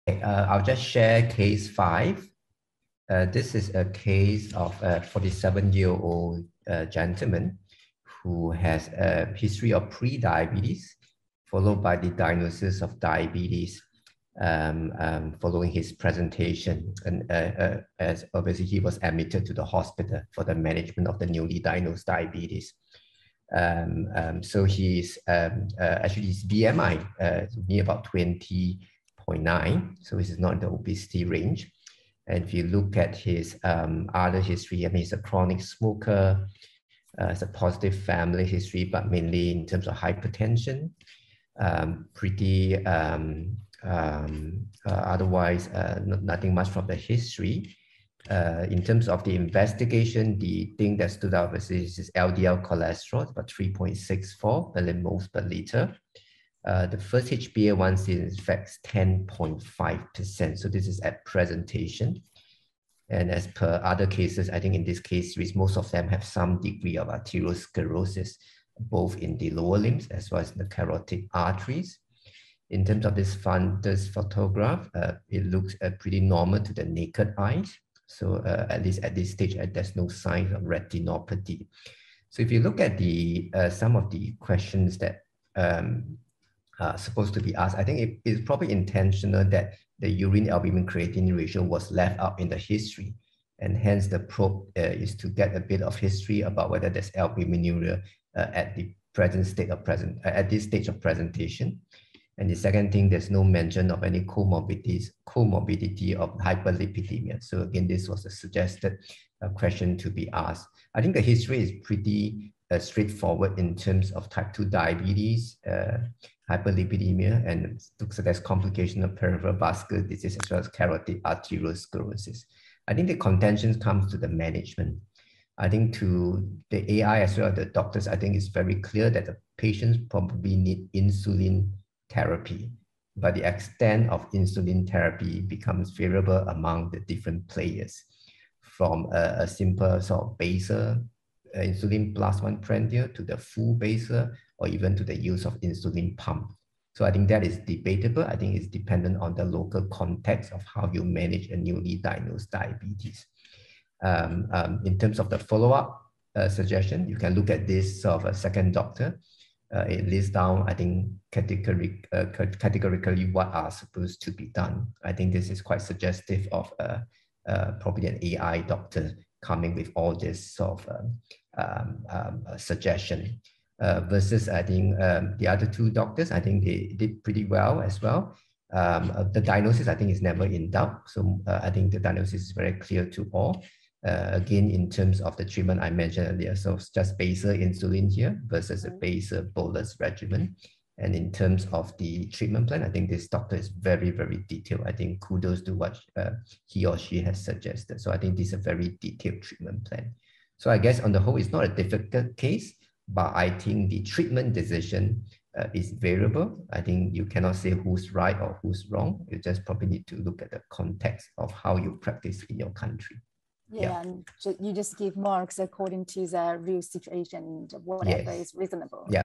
IDF Virtual Congress 2021: International Human-Machine Competition in Diabetes Management - Part 5
Case 5 review by judge Bee Yong Mong, Head and Senior Consultant in the Department of Endocrinology at Singapore General Hospital.